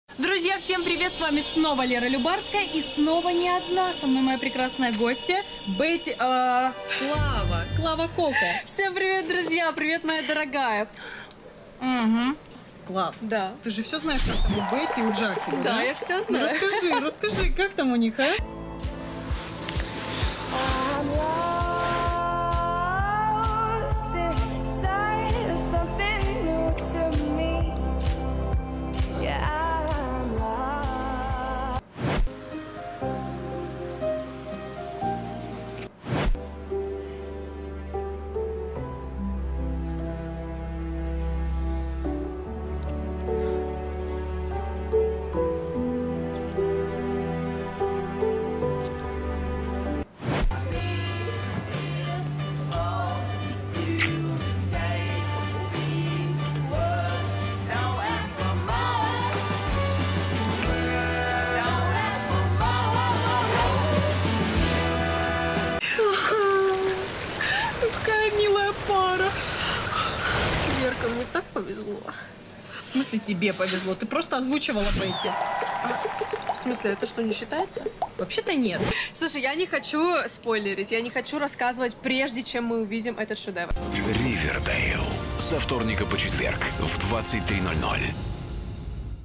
Любарская и Клава Кока шипперят героев Ривердэйла
Блогеры Лера Любарская и Клава Кока шипперят главную пару сериала Ривердэйл - Багхед (Бетти Купер и Джагхеда Джонса).